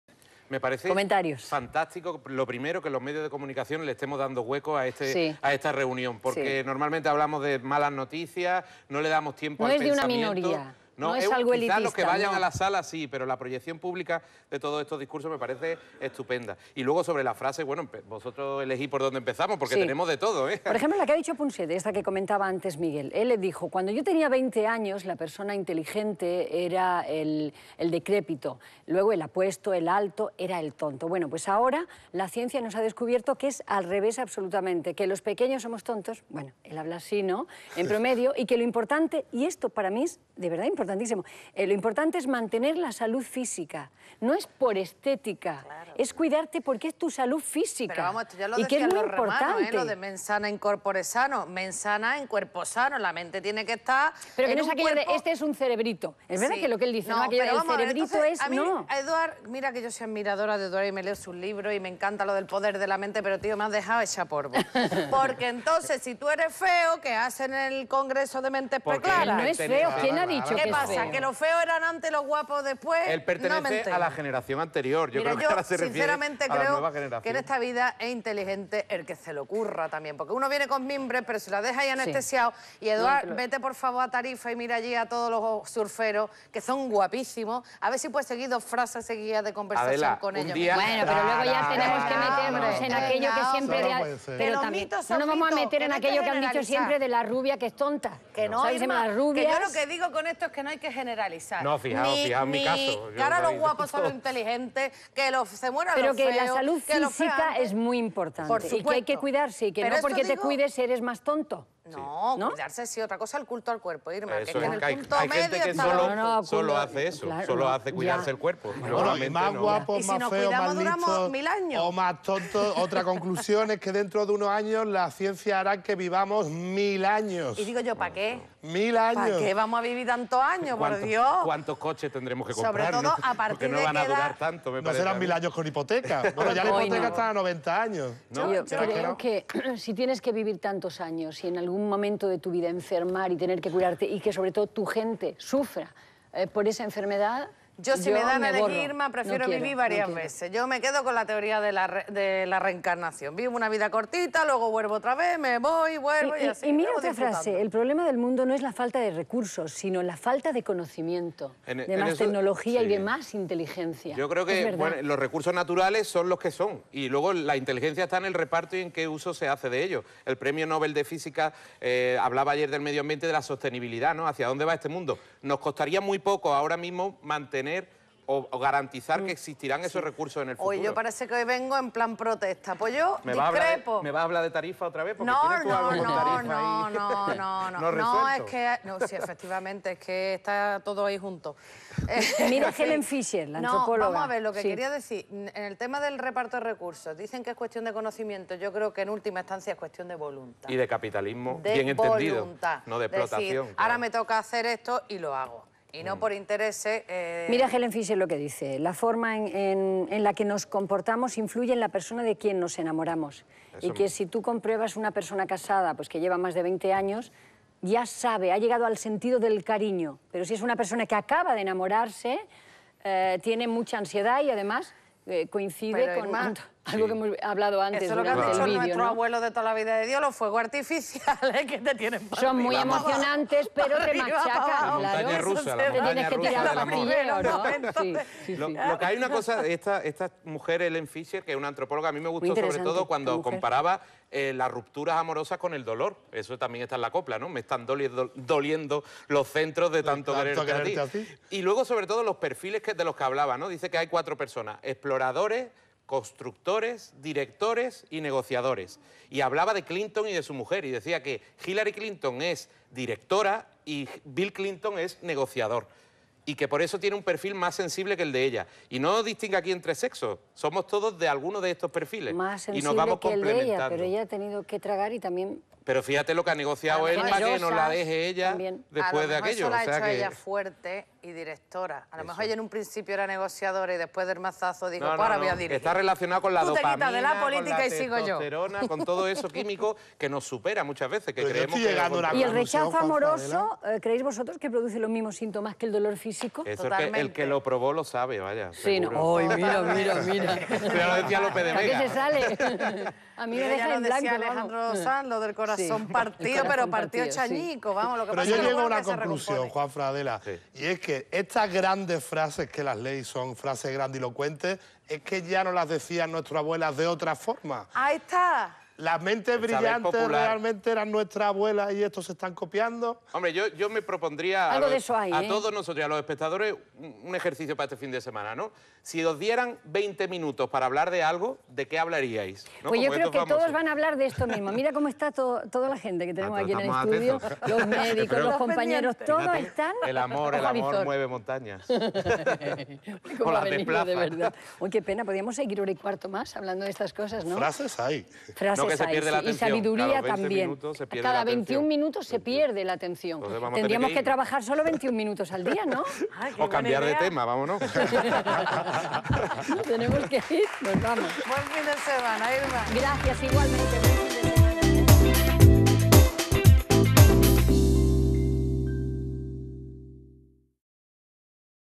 Mentes brillantes Actualidad Mira la vida
Intenso debate sobre los contenidos del congreso de "mentes brillantes" en el que participa Punset